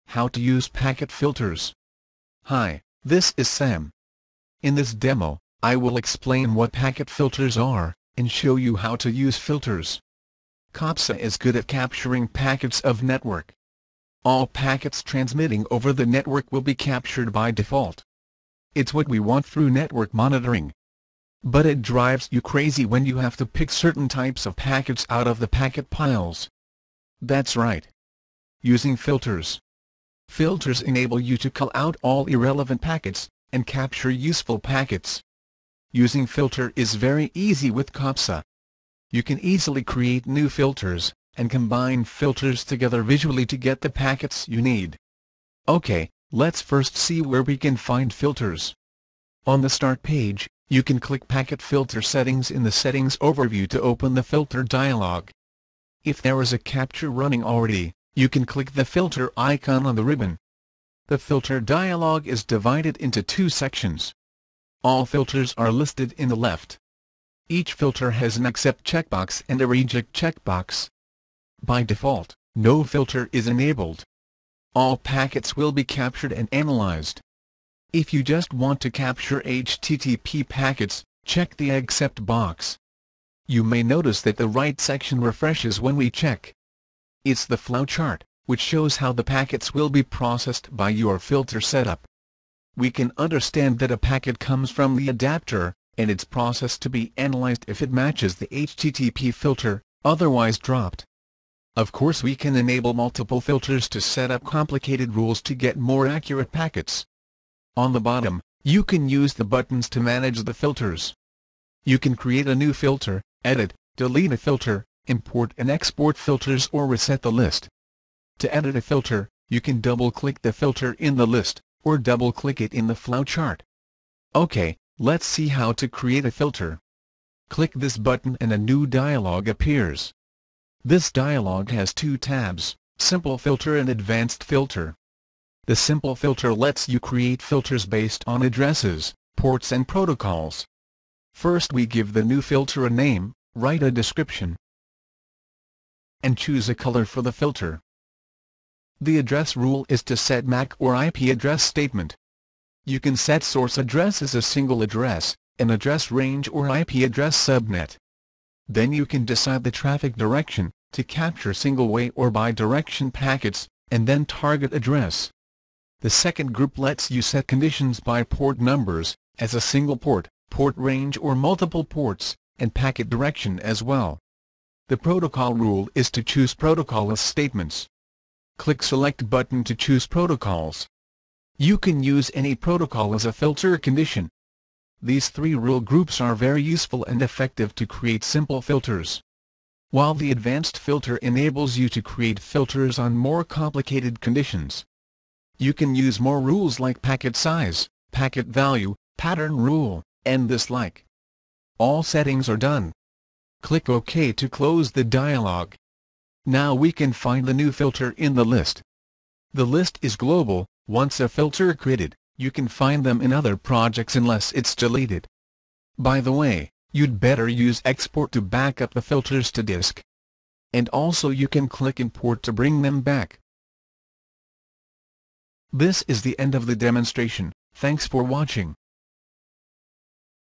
How to Create a Filter in Capsa
This video introduces how to create capture filter in Colasoft Capsa to only capture traffic of interest.